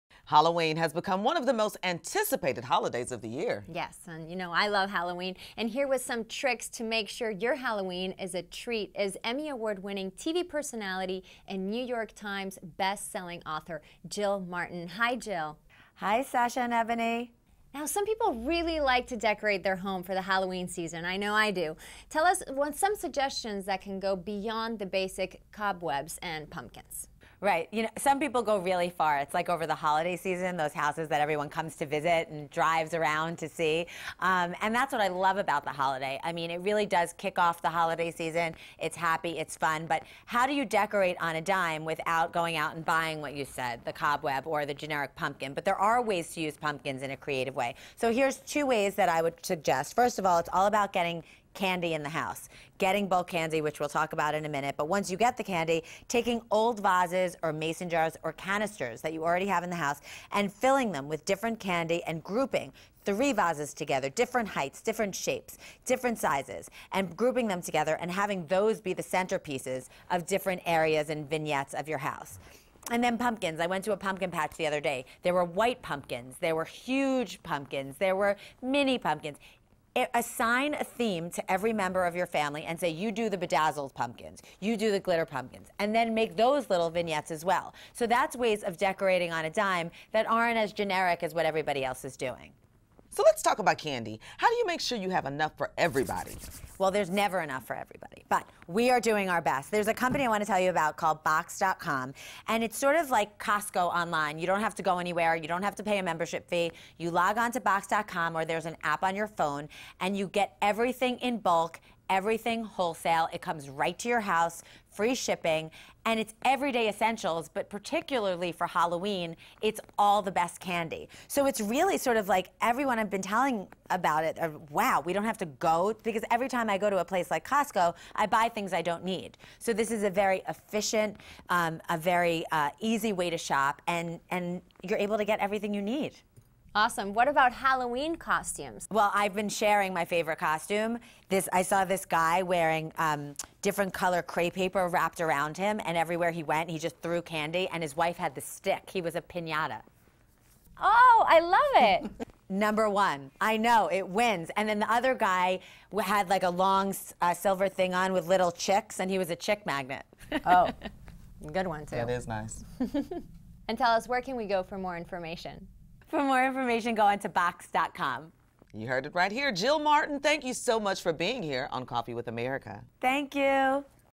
How to Plan For The Most Wicked Halloween Season Ever
Emmy-award winning TV personality and New York Times best-selling author Jill Martin shares her secrets on finding the best steals and deals to ensure a terrifyingly fabulous Halloween.